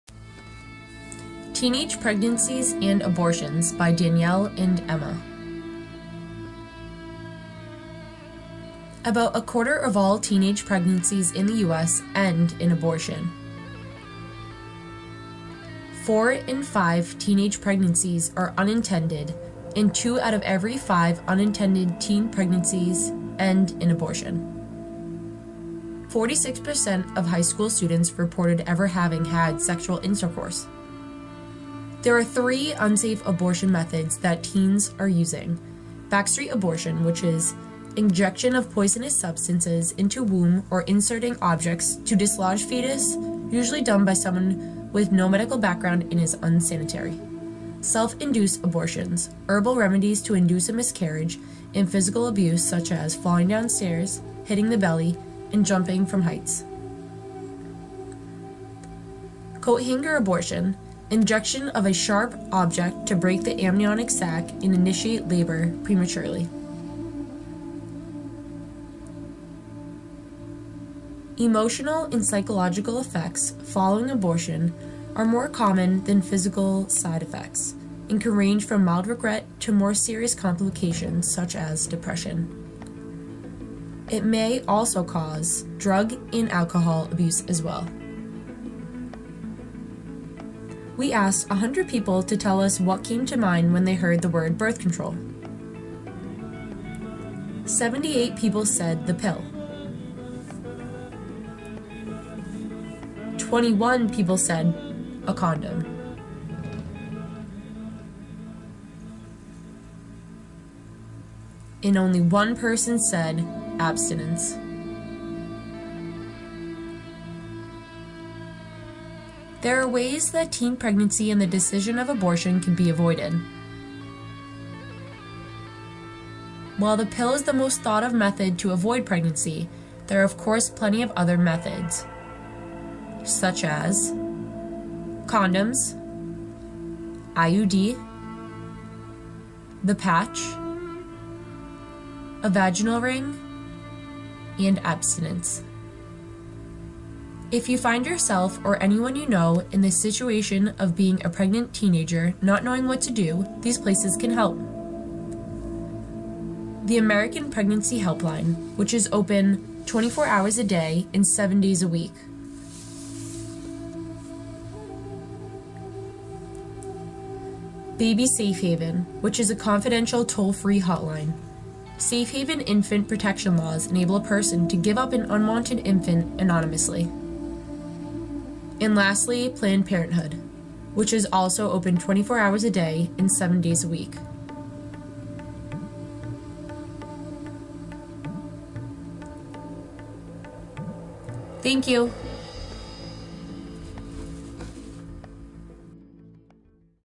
Teenage Pregnancies and Abortions
Our movie spreads awareness of teenage pregnancies and abortions, and the rising rates. It describes the unsafe methods, psychological and mental effects of abortions as well. We also asked 100 people a question regarding birth control. At the end are places to those in this situation to turn to.